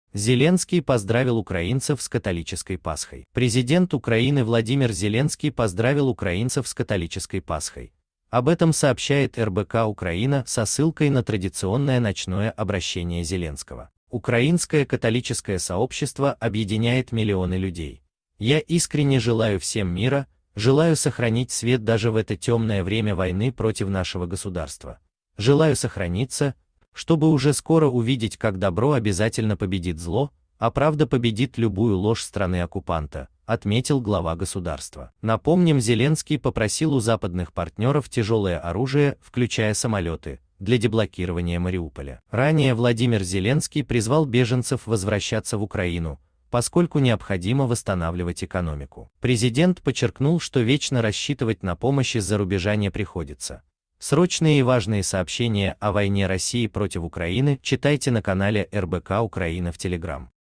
Зеленский поздравил украинцев с католической Пасхой.
ВладимирЗеленский Пасха Президент Украины Владимир Зеленский поздравил украинцев с католической Пасхой. Об этом сообщает РБК-Украина со ссылкой на традиционное ночное обращение Зеленского. "Украинское католическое сообщество объединяет миллионы людей. Я искренне желаю всем мира, желаю сохранить свет даже в это темное время войны против нашего государства. Желаю сохраниться, чтобы уже скоро увидеть, как добро обязательно победит зло, а правда победит любую ложь страны-оккупанта", - отметил глава государства. Напомним, Зеле